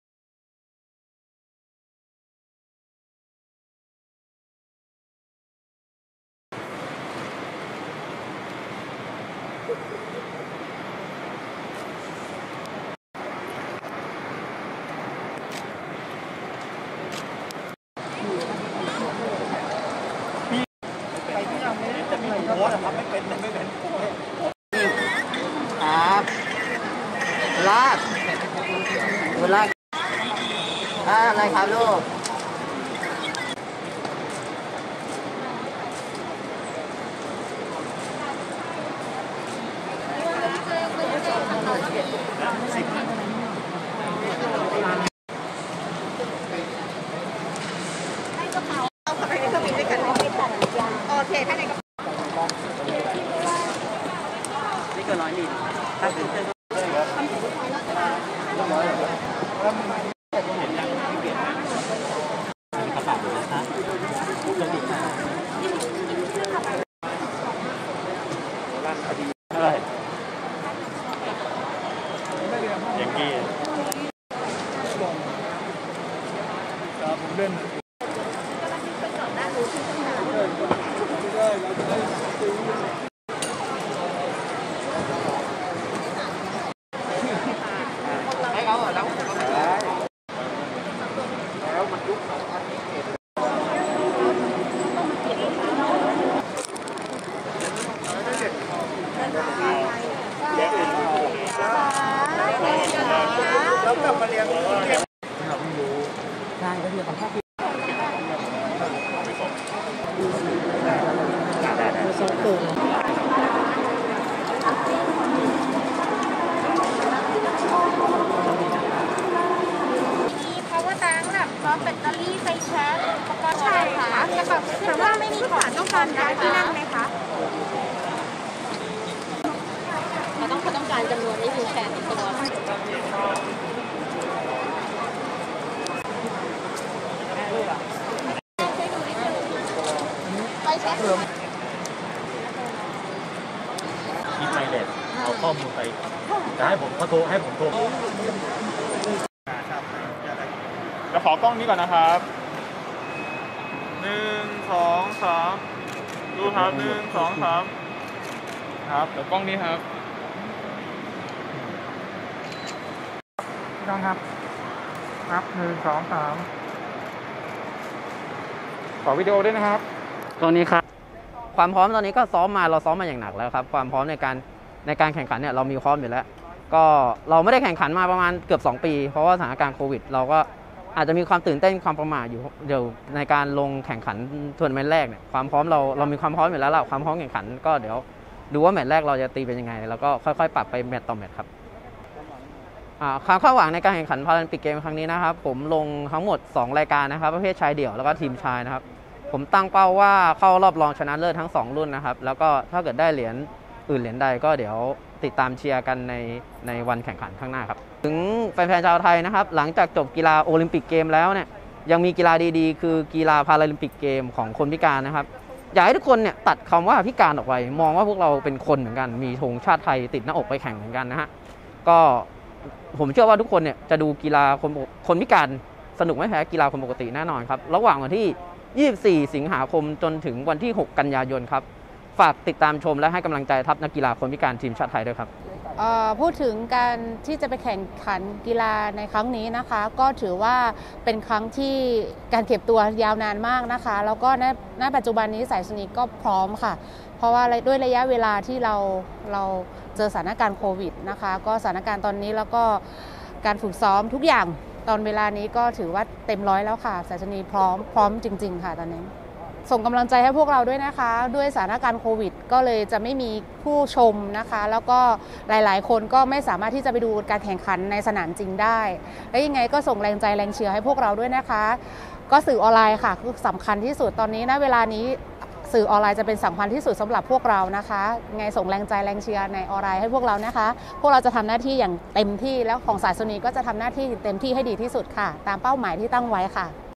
ภาพข่าวทัพนักกีฬาพาราลิมปิกไทย ชุดแรกเหินฟ้าสู่ประเทศญี่ปุ่นเมื่อวันที่ 17 สิงหาคม 2564
สัมภาษณ์1.
1.รุ่งโรจน์ ไทยนิยม
นักเทเบิ้ลเทนนิสพาราไทย
2.สายสุนีย์ จ๊ะนะ
นักวีลแชร์ฟันดาบ นำทัพนักกีฬาทีมชาติไทย